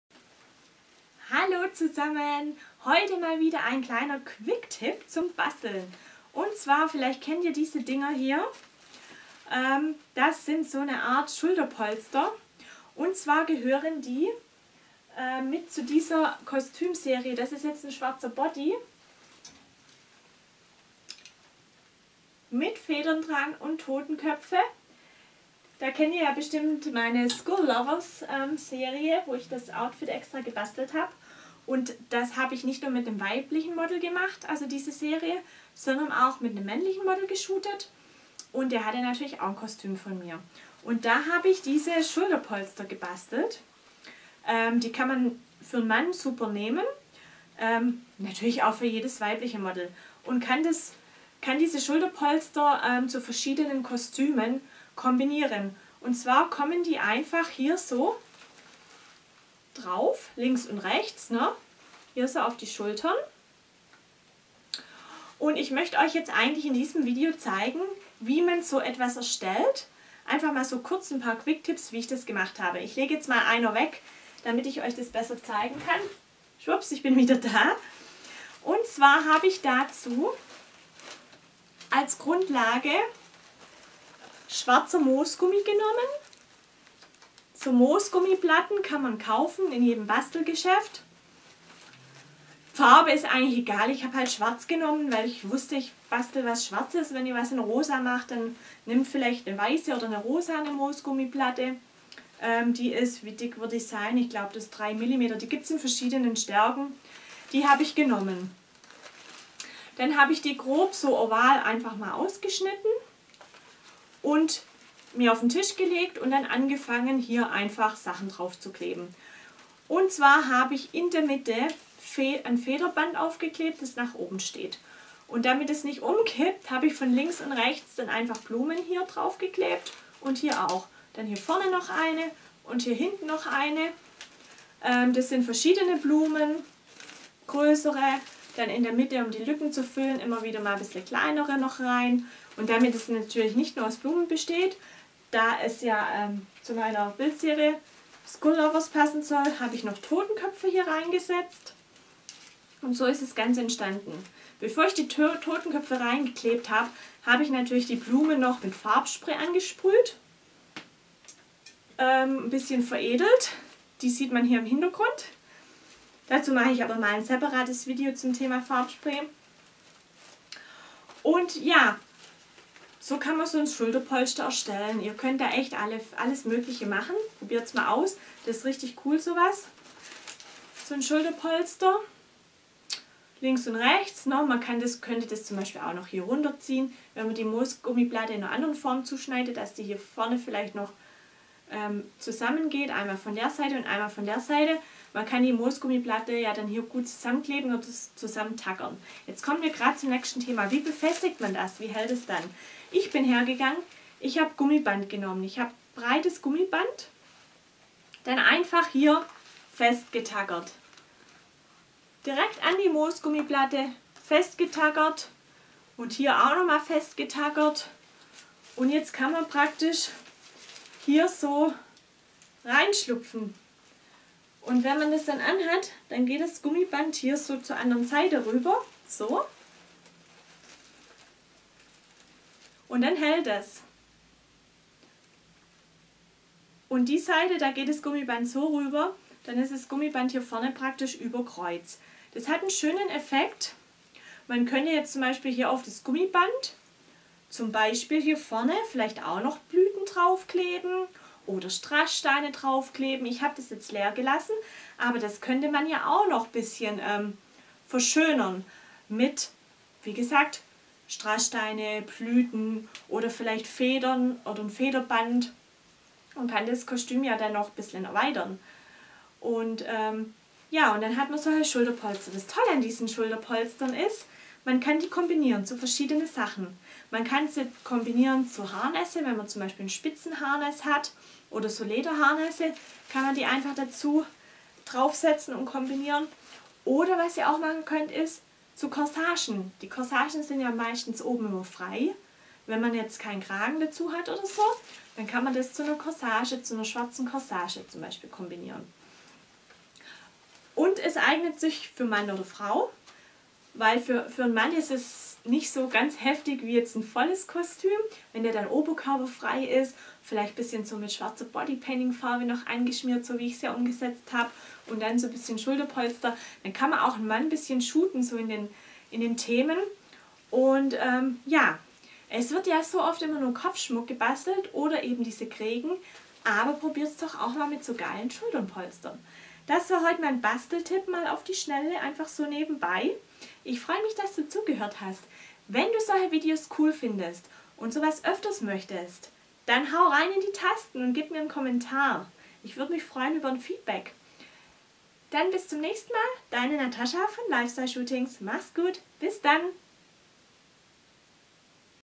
Bastelwahnsin - Schulterschmuck
Ich hab ein kleiner Basteltipp für euch. Habt ihr schon mal einen Headdress und vielleicht sogar auch schon mal einen Kragen gebastelt? Wie wär’s denn dann auch mal damit ???